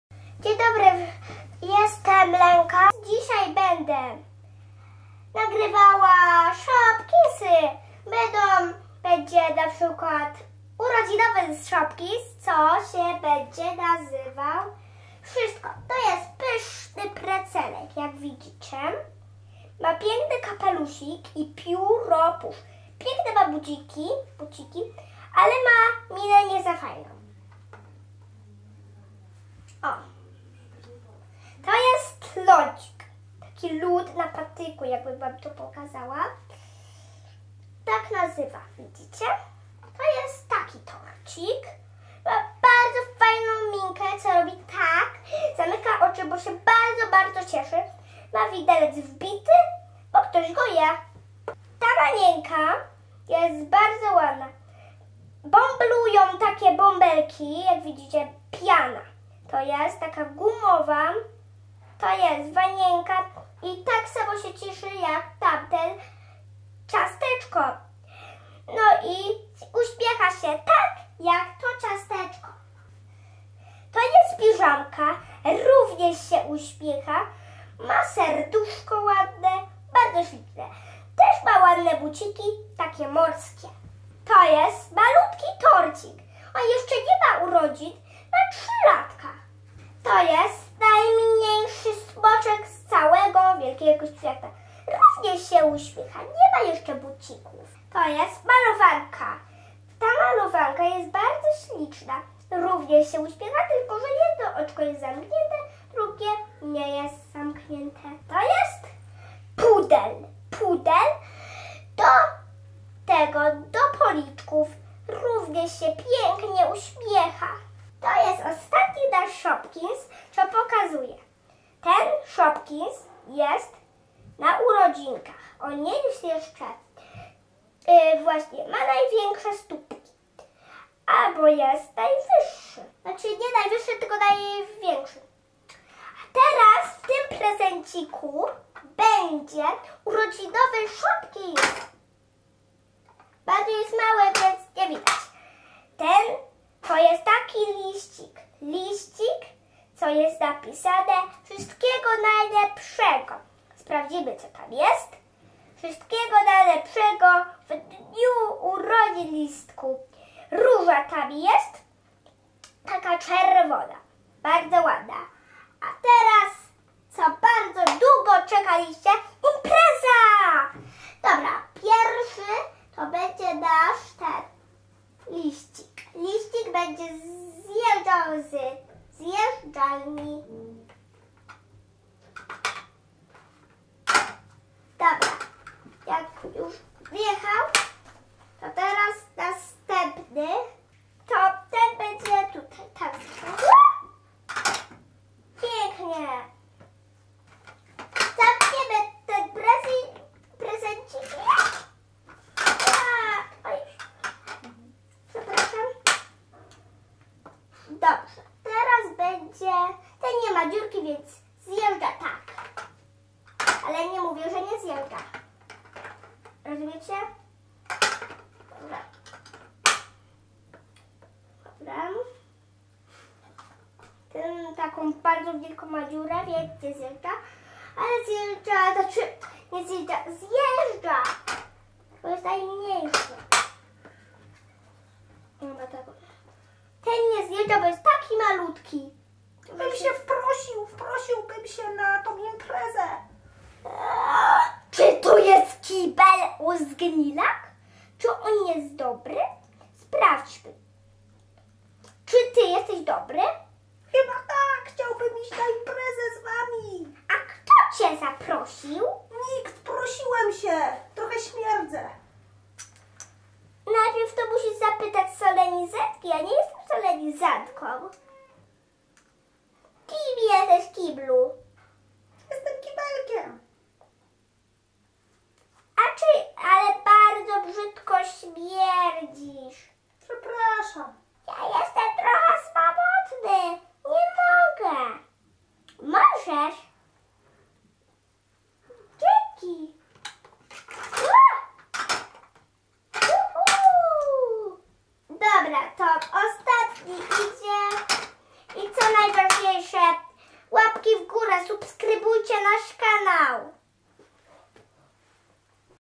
Shopkins - zabawki zjeżdżalnia
Lenka bawi się swoimi shopkinsami.